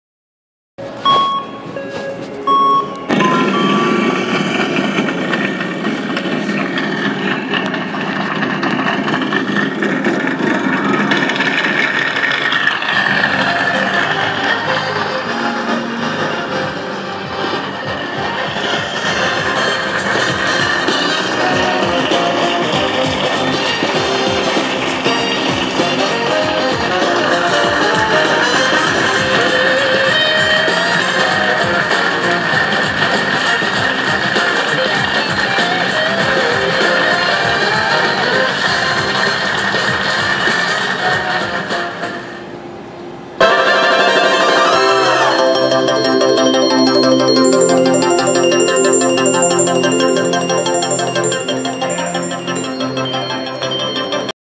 High Limit Wheel of Fortune Bonus Jackpot as it Happens Handpay
I got to spin the high limit wheel of fortune bonus game.  I caught my jackpot on film as it happened!